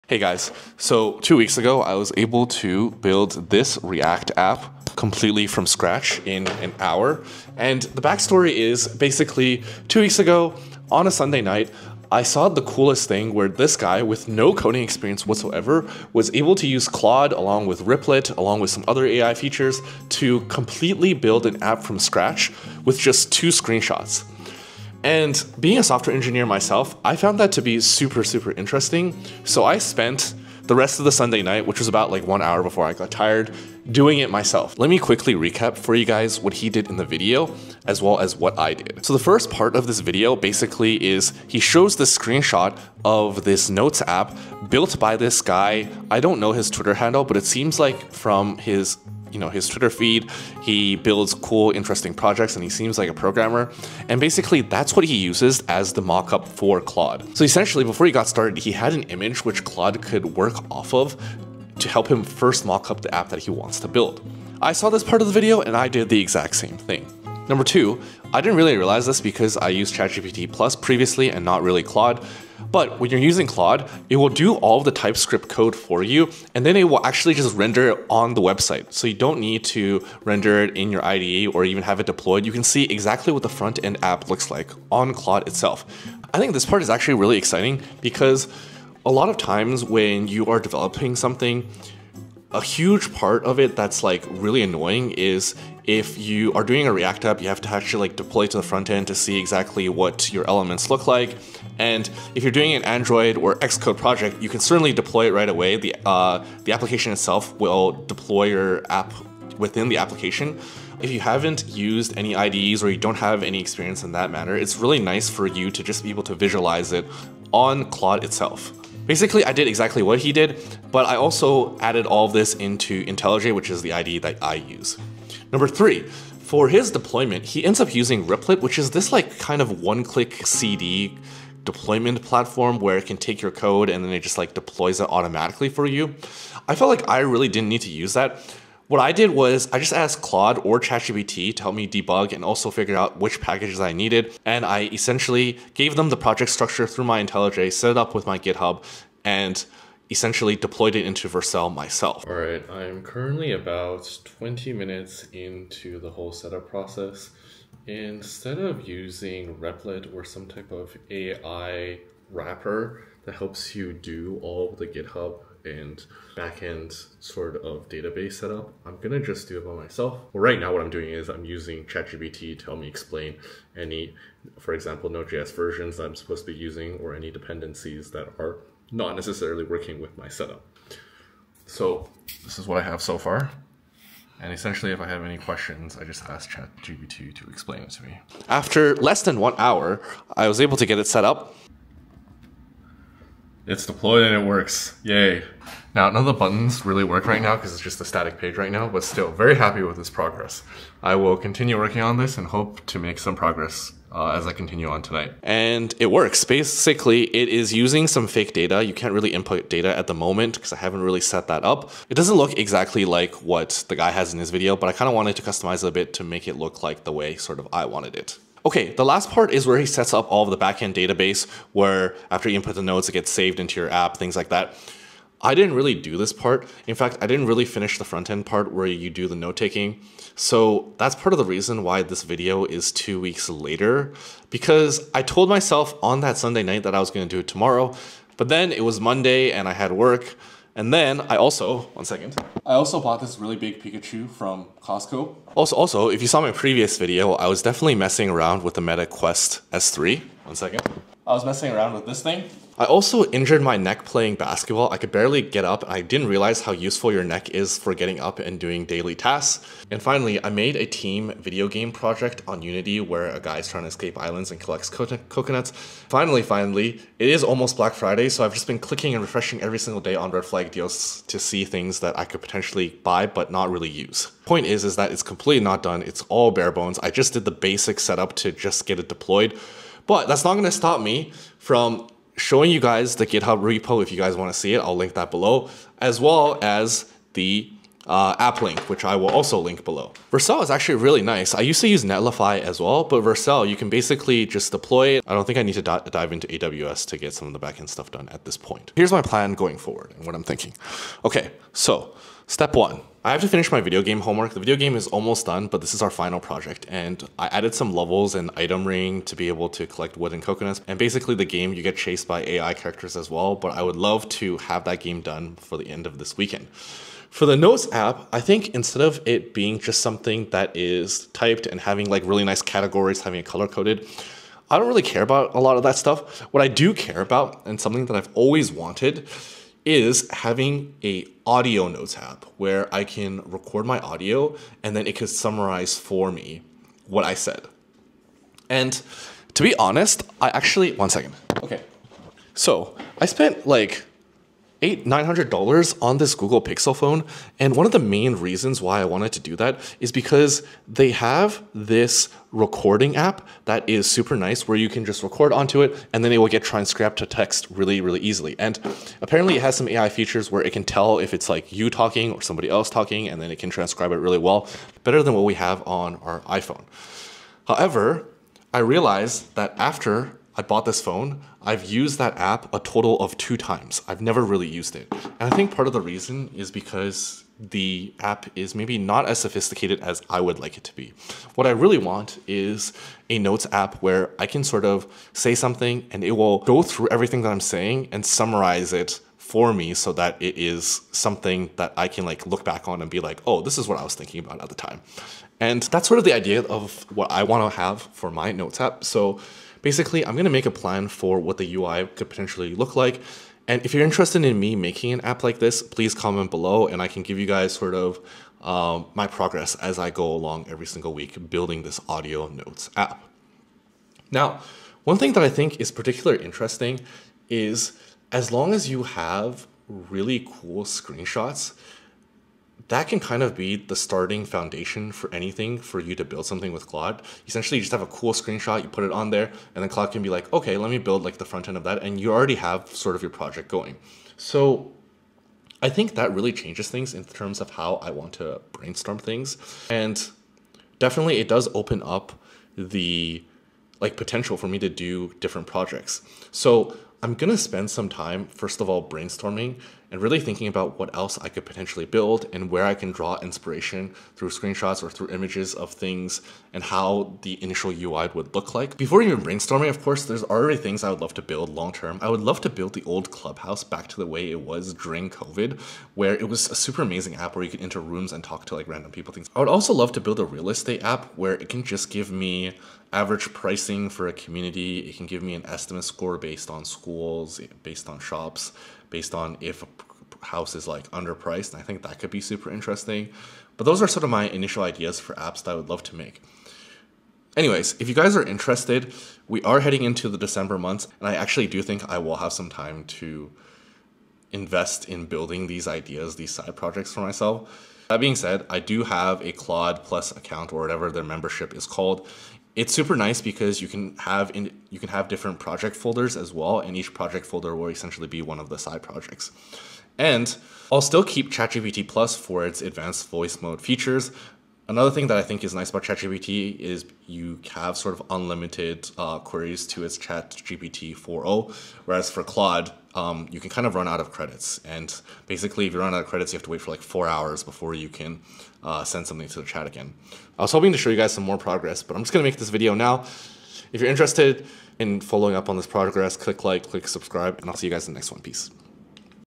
I Built a Notes Web App With Just a Screenshot and Claude in 1 Hour
Sorry I didn't put more effort in the notes app LOL, Black Friday is distracting
Github repo (sorry I wish I put more effort before making the video lol)

Hey guys, so two weeks ago I saw the coolest thing which was this guy had no coding experience build an app completely with claude with 2 screenshots.

Being a software engineer myself, I found that to be super interesting so I spent the Sunday doing it myself.

Let me recap with you guys what I did.

The first part of his video he shows a screenshot.

- This is what he uses to give claude a first mock up of the app he wants to build

Second, he creates starts using claude to do the front end react code:

- this part is super easy now because you can literally see the renderings on claude

Third

- For his deployment he ends up using Replit, kind of like a one click deployment platform
- I felt like I didn’t really need to do that, sure I could but Setting up the packages the dependencies, using vercel to easily deploy I didn’t feel like I needed another monthly subscription to help me do that
    - So I did it all in an hour

Then the third part is the backend database stuff. While I have not really done. So this video is literally 2 weeks late, because I was like I’m going do it tomorrow. Then I ended up being really busy for work, I bought this huge pikachu from Costco, I was messing around with meta quest 3s that I will be returning, I injured my neck playing basketball, I made a team video game project on unity and it’s almost black Friday so I have been clicking refreshing every day on red flag deals.

My point is it is not done, but I will show you the github and deployed to vercel link anyways. You can also go to that guys video to see exactly how a notes app is built. But I don’t want to just build a notes app.

So here’s the plan going forward :

1. I’ve got to finish my video game homework: I added levels recently and basically the game is to collect wood and coconuts while being chased by ai characters.
2. For the notes app I would love to make it something useful for me. Something I’ve always wanted to do is have a really nice voice recording feature that summarizes what I’m saying.
    1. I literally bought this pixel phone to be able to do that but I’ve actually been to lazy to use it. So I’m going to try to make my note app an audio notes app
3. I like the idea of going off a really cool screenshot and starting as that with the foundation as what we use to build something
    1. So I’m going to spend some time brainstorming and then really thinking about what else I want to build
        1. Before brainstorming further I already some things I would love to build long term. I would love to build clubhouse back to the way it was before covid, that was an amazing app.
        2. I would love to build real estate app that can help me figure out average pricing in communities for statistical collection